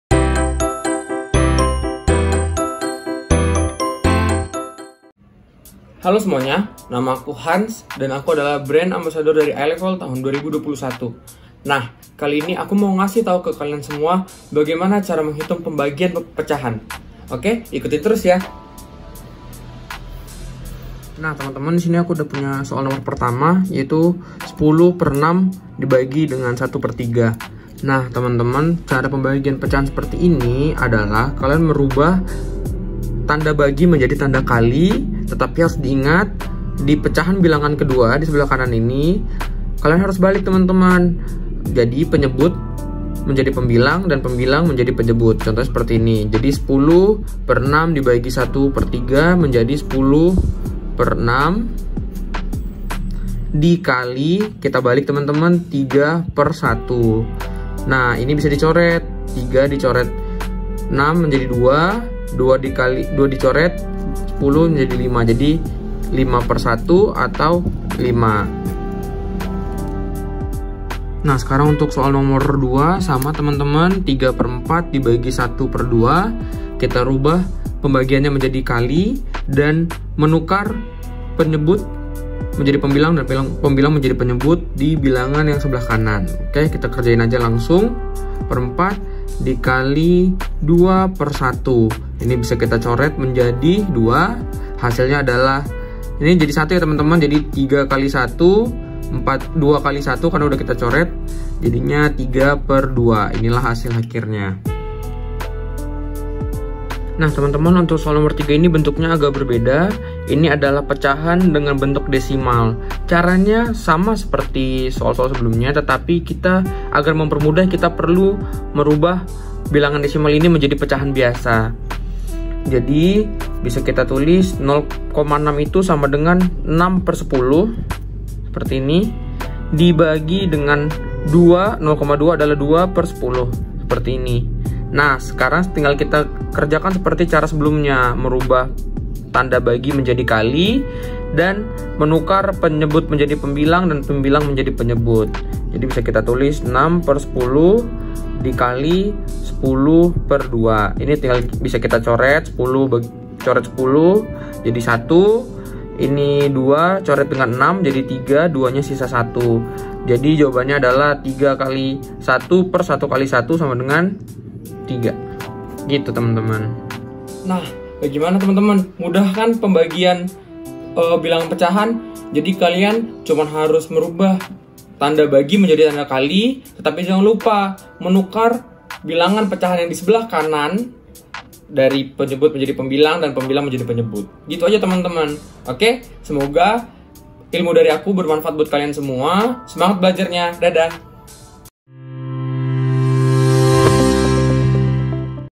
Menghitung Pembagian Pecahan - Belajar Bersama Eye Level
Kali ini kita akan belajar menghitung pembagian pecahan bersama Eye Level Ambassador, Kak Hans!
Untuk yang masih bingung bagaimana cara menghitung pembagian pecahan, yuk simak videonya!

Dan pastinya, Belajar Matematika dan Bahasa Inggris lebih mudah dan menyenangkan bersama Eye Level!

Cek Instagram kita untuk update promo menarik, informasi lebih lanjut tentang les di Eye Level!